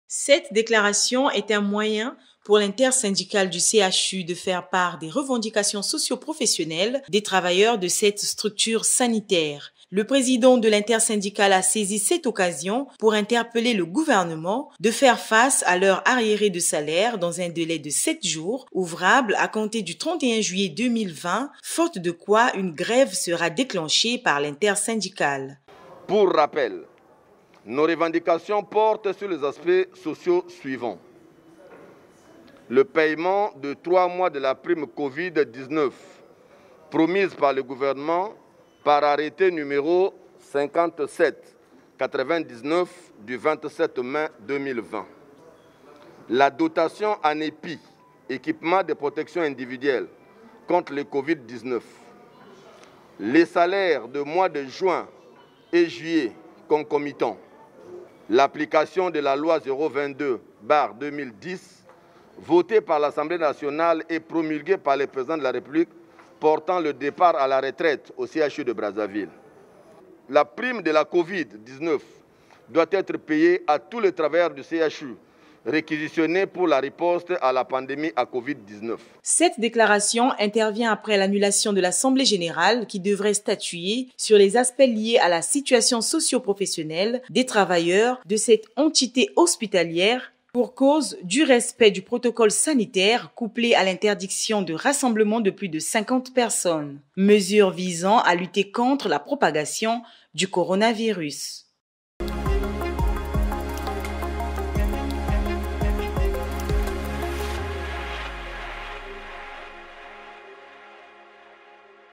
Menace de grève au CHU de Brazzaville
Le président de l’intersyndicale du Centre hospitalier universitaire de Brazzaville (CHU-B) a, dans une déclaration, revendiqué les droits socioprofessionnels des travailleurs de cette structure sanitaire.